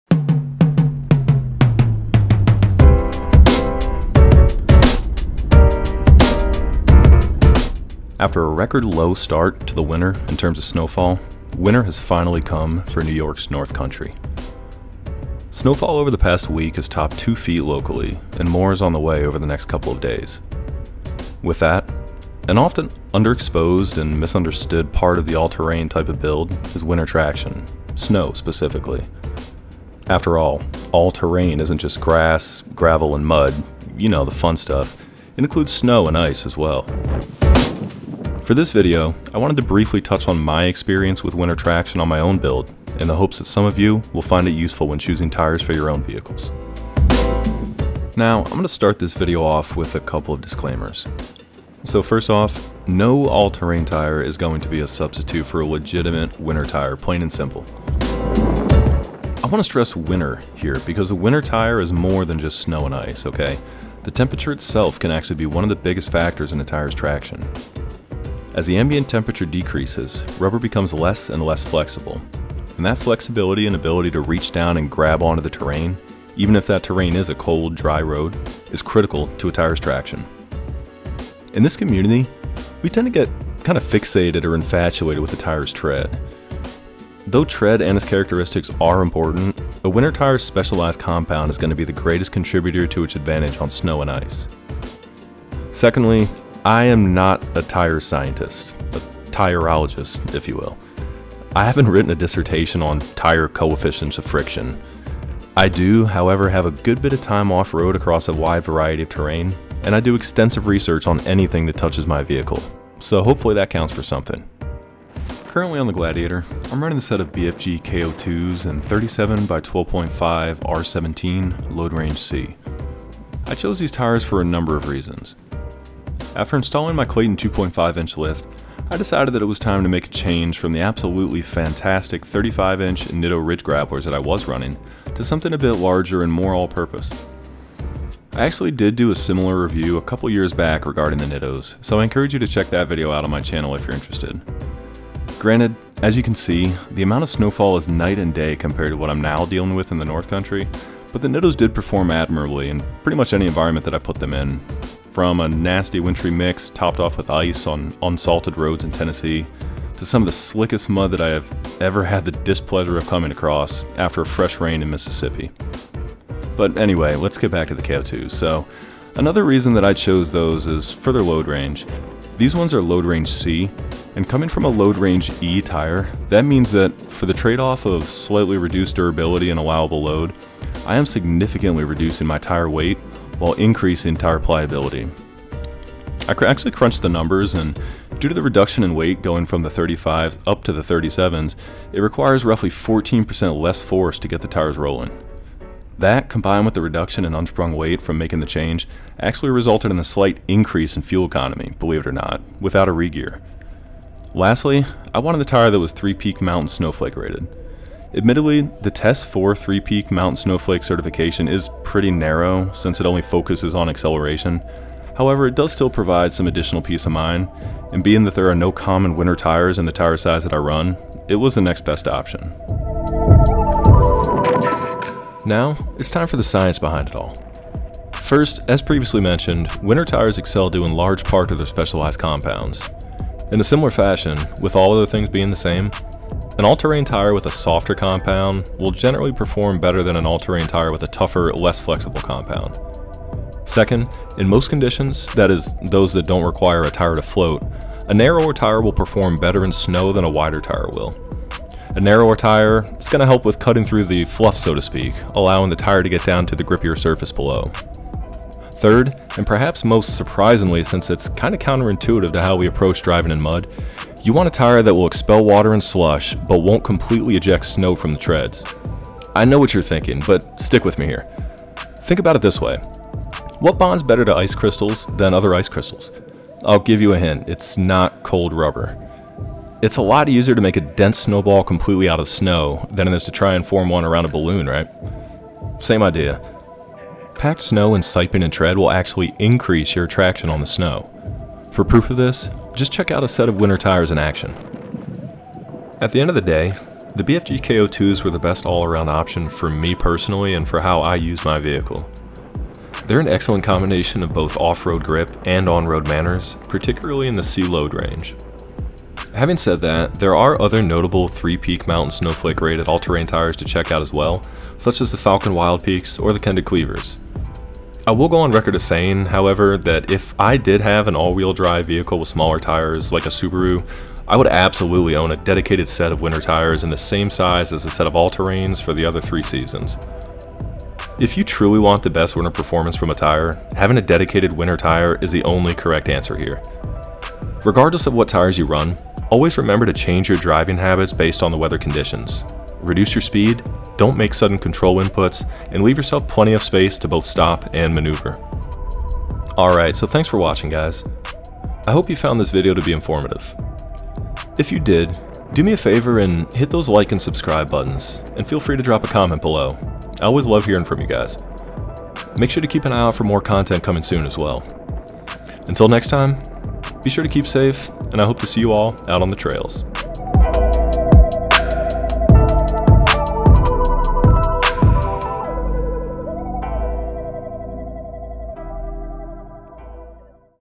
Tech Talk | Tires - Winter Traction. All-terrain Tires (BFG KO2) vs. Winter Tires
The second installation of my tech talk on tires digs a bit deeper into what makes for a good winter tire, and how winter traction translates to all-terrain tires, such as the 37x12.50R17 BFG KO2s that I have on my Jeep.

00:00 Introduction
00:47 Setting the Scene
01:55 BFG KO2s
03:44 The Science Behind Winter Tires
05:30 Conclusion